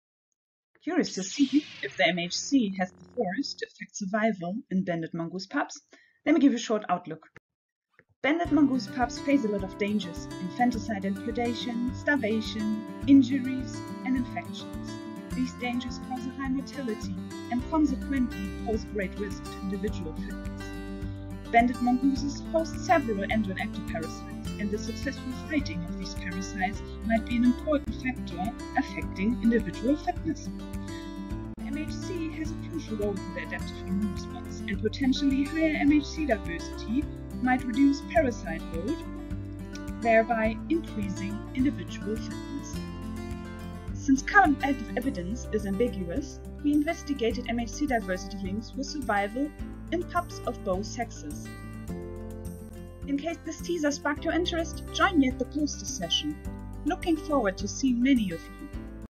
MHC diversity affects pup survival in male banded mongooses
A poster teaser for the symposium, Research Frontiers in Animal Behavior & Parasitism Hosted by the Center for the Ecology of Infectious Diseases at the University of Georgia, May 20th and 21st, 2021

Poster presented by Nadine Schubert, PhD Student, Bielefeld University